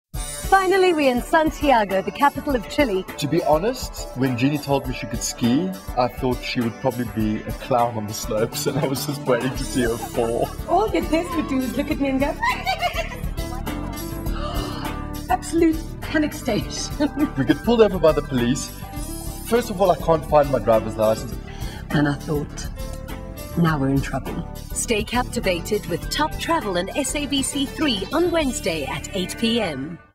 Santiago Chile, Episode 5
Upon arriving in South America's fifth-largest city, Santiago in Chili, Jeannie and Janez go skiing around the dazzling backdrop of the Andes Mountains.